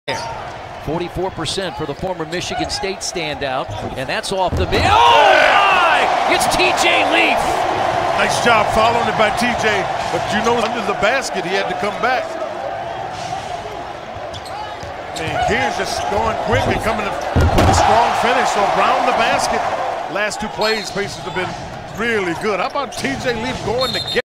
T.J. Leaf's Massive Putback Dunk vs. Spurs [23.11.18.]
Get your NBA GOAT SportzCases here!

Indiana Pacers T.J. Leaf's Massive Putback Dunk vs. Spurs [23.11.18.]